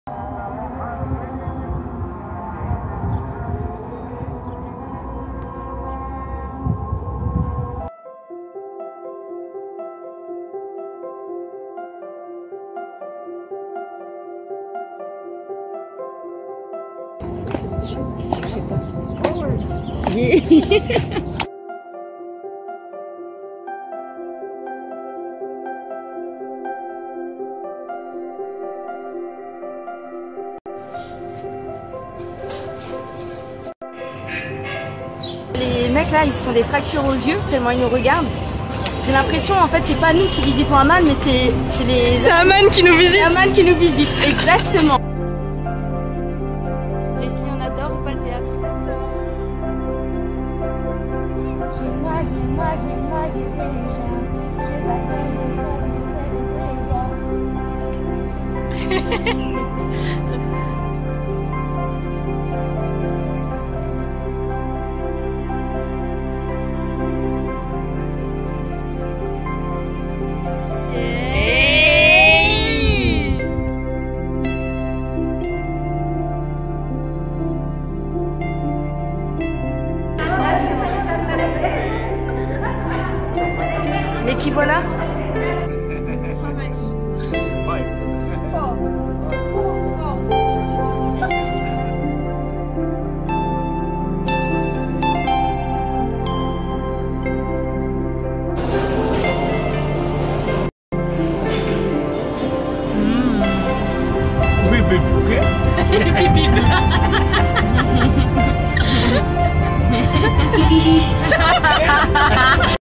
JORDANIE 2018 - Jour 2 : Amman
Visite de Amman Downtown, Jabal Al Weibdeh, le théâtre romain et la citadelle.

Pour plus d'infos sur ce roadtrip de 2 semaines en Jordanie, RDV sur mon blog